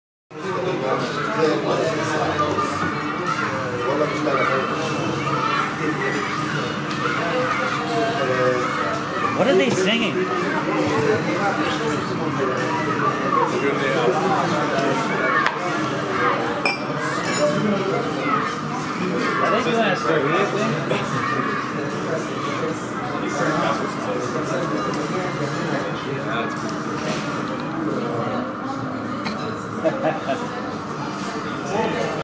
School Children in Aleppo
We were grabbing a bite in Aleppo and saw these happy faces walk past the cafe. Filmed April 2018